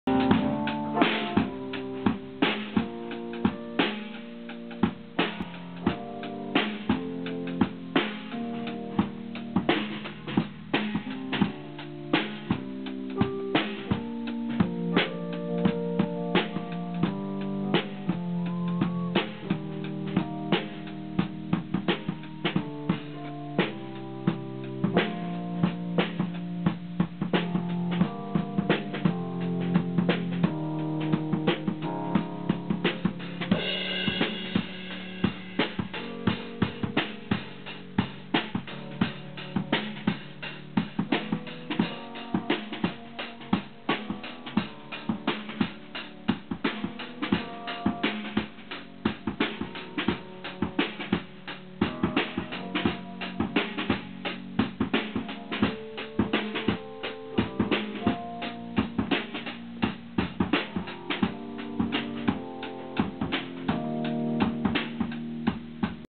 Reef Jamming @ Cherokee Studio
Killing time at the studio between mixes was a hard thing to do. This is a short clip of Reef jamming with Andrew.